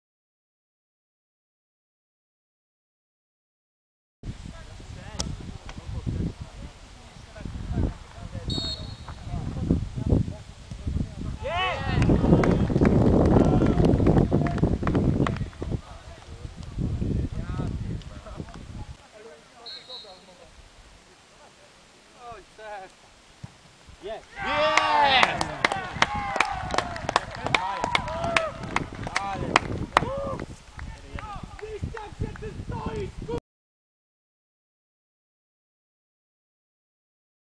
ZGODA Chodecz 6- 3 SADOWNIK Waganiec PP
Dwie bramki z meczu PP - R.Dekrewicz/k/ i Jacek Majewski/w/ - rozegranego w Chodczu - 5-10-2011 r.